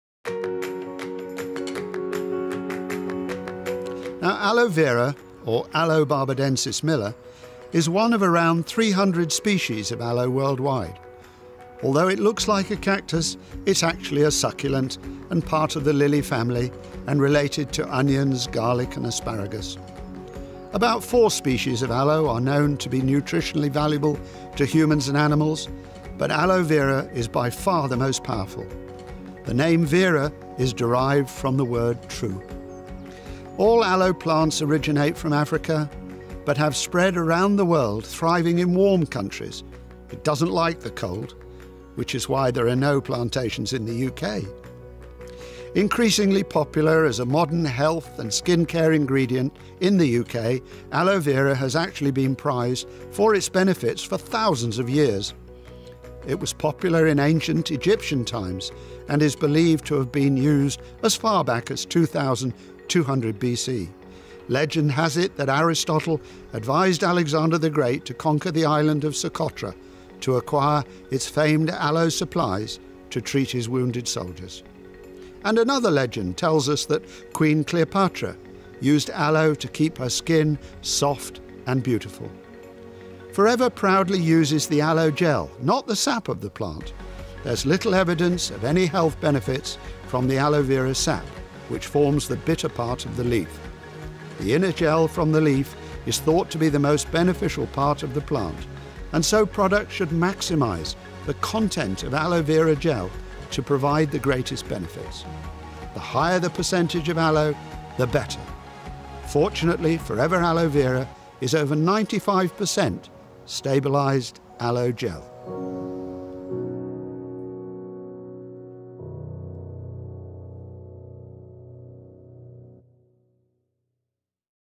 All About Forever Living's Aloe The History of Aloe
The aloe vera plant has been known and used for centuries for its health, beauty,medicinal and skincare properties.Humans have been recording the benefits of aloe vera for 2,500 years,and there are examples of aloe vera in Egyptian tombs,the bible and in famous ancient Greek,Roman,Chinese and Indian herbals texts.Aloe vera’s many properties and its value has not diminished over time.Cleopatra’s favourite beauty product was indeed aloe vera,which she used to keep her skin and hair looking radiant.In fact,many Eastern women have used aloe vera as a beauty secret for centuries.Why don’t you feed your skin from within with Forever Aloe Vera Gel?